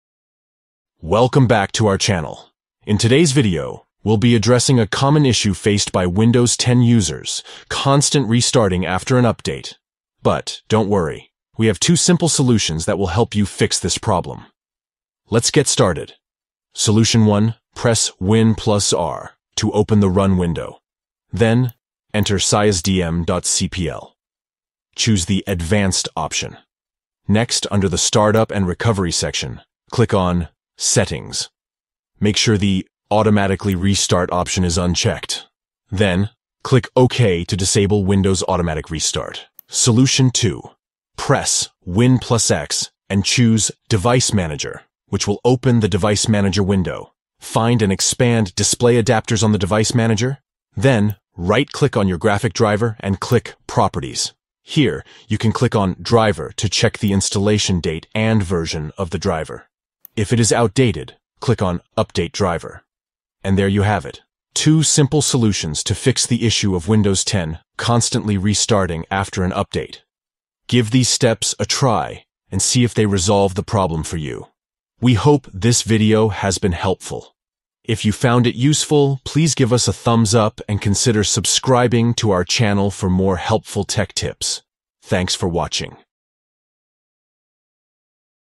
How to Fix Windows 10 Keeps Restarting after Update
In this video, we will show you how to fix the issue of Windows 10 constantly restarting after an update. Follow these steps:

1. Press Win + R to open the Run window. Then enter sysdm.cpl.
2. Choose the Advanced option. Next, under the Startup and Recovery section, enter Settings.
3. Make sure the Automatically restart option is unchecked. Then click OK to disable Windows automatic restart.

Solution 2:

1. Press Win + X and choose Device Manager, and you will enter the Device Manager.
2. Find and expand Display adapters on the Device Manager. Then right-click on your graphic driver and click Properties.
3. Here you can click Driver to check the installation date and version of the driver. If it is outdated, click Update Driver.

Learn how to fix the issue of Windows 10 constantly restarting after an update with these simple steps. Watch the video for a detailed walkthrough.